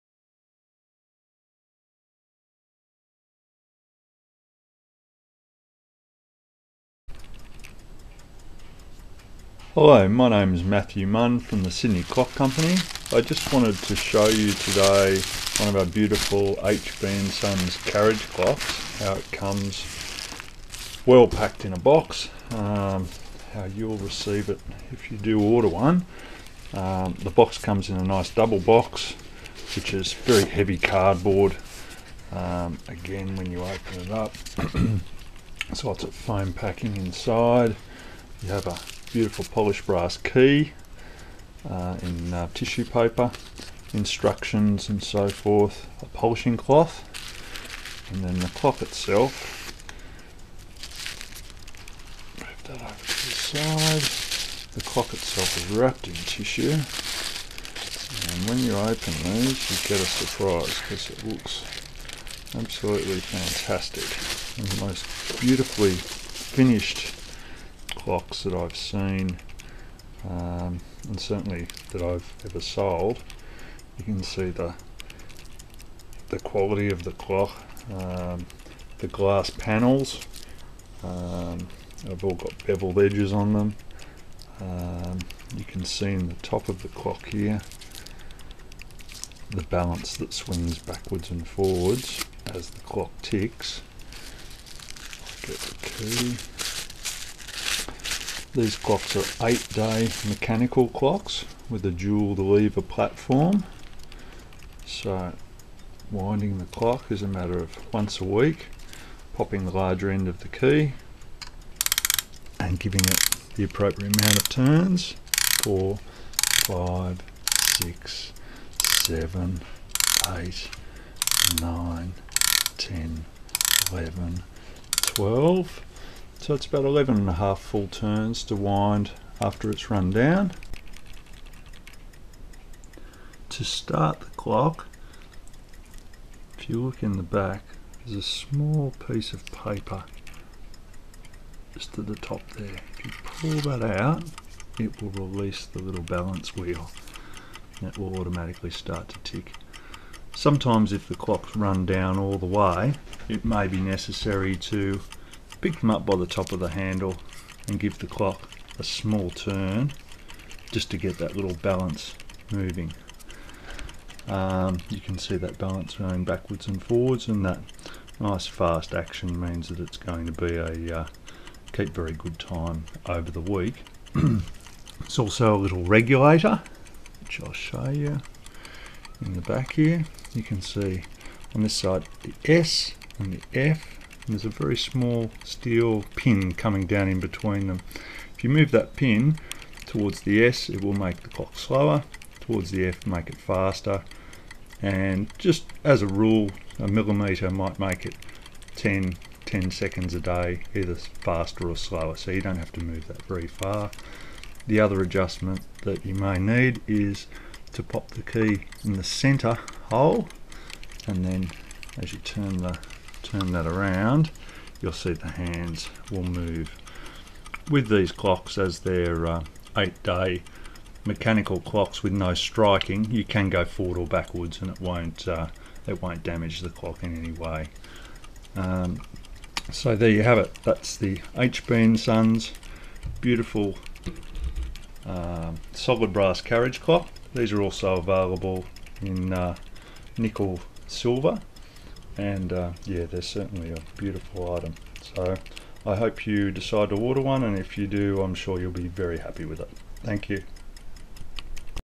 HB & Sons 8 Day Mechanical Carriage Clock by Matthew Munn. Available from the Sydney Clock Company.
HB & Sons WM4 8 Day Mechanical Carriage Clock by Matthew Munn. Available from the Sydney Clock Company.